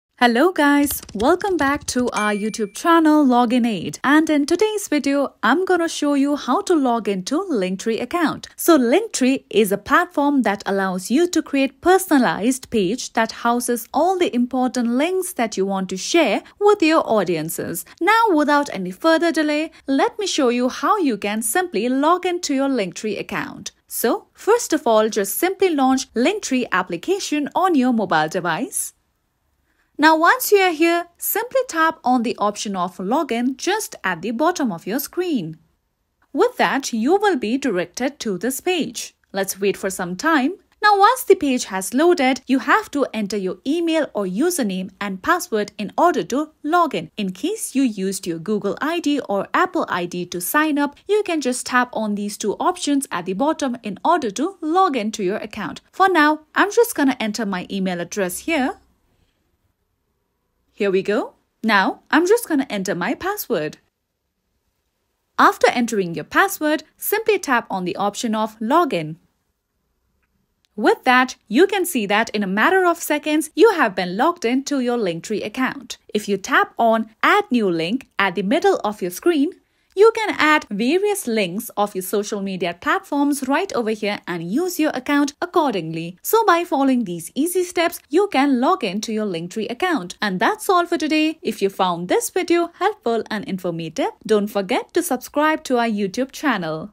How To Login Linktree Account 2024 | Linktree App Sign In Tutorial | Link In Bio Tool
Learn how to log in to your Linktree account with this step-by-step tutorial. Whether you're a content creator, business owner, or social media user, this guide will show you how to easily access and manage your Linktree account to create custom landing pages and optimize your social media presence.